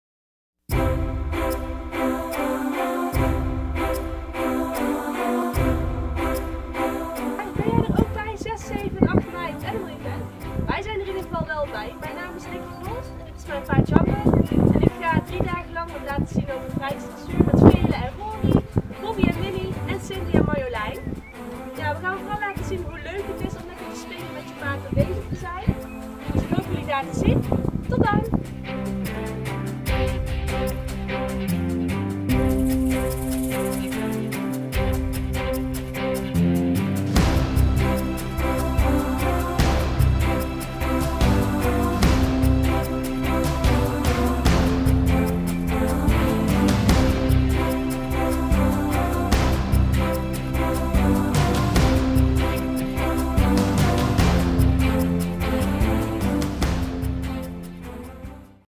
Promo Animal Event 2016
6, 7 en 8 mei is Nikki van Olst, met haar team bestaande uit Veerle met Rori, Bobbi met Winnie en Marjolein met Cindy aanwezig op Animal Event om te laten zien hoe leuk 'spelen met je paard' kan zijn!